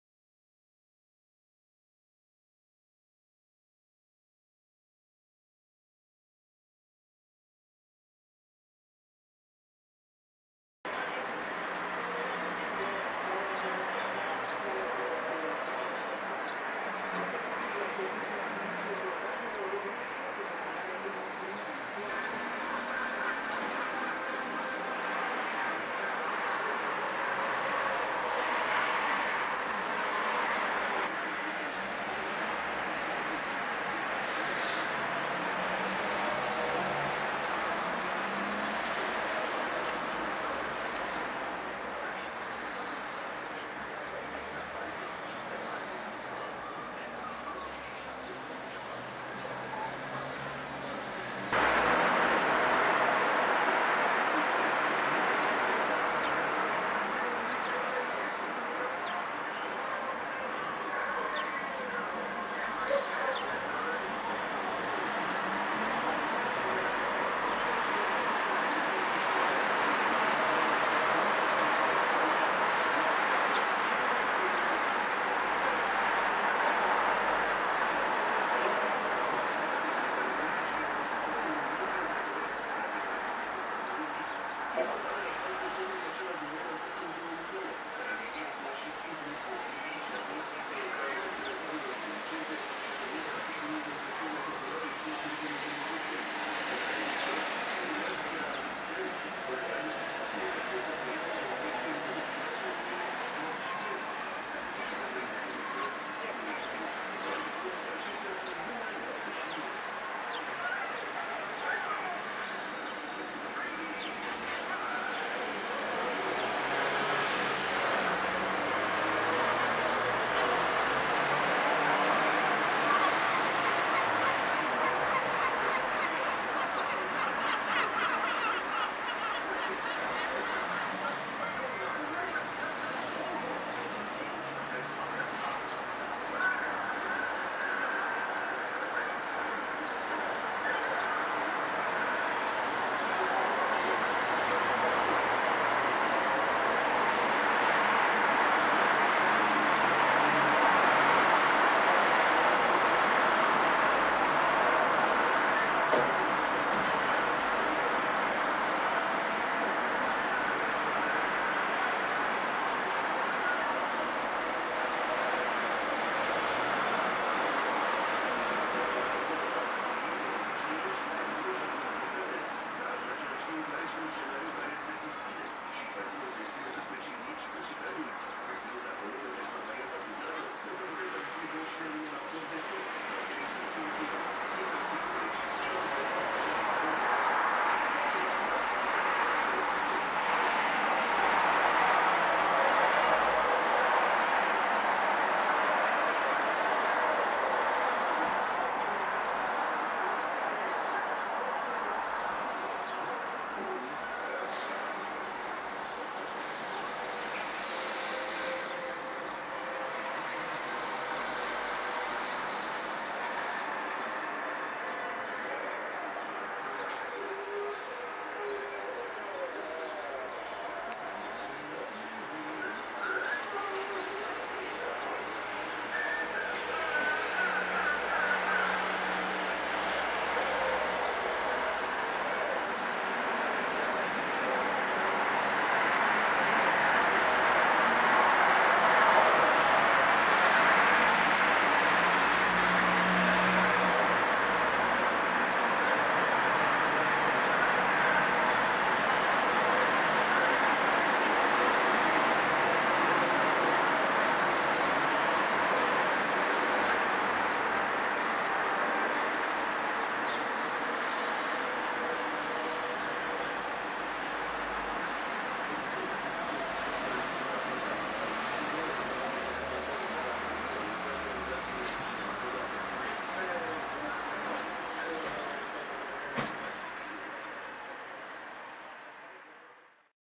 Pranz in familia de albatrosi
Un pranz in familia vecinilor nostri , ALBATROSII
Suceava, 15 mai 2015 - o familie de albatrosi cu doi pui frumosi, vecini cu noi, i-au pranzul acasa.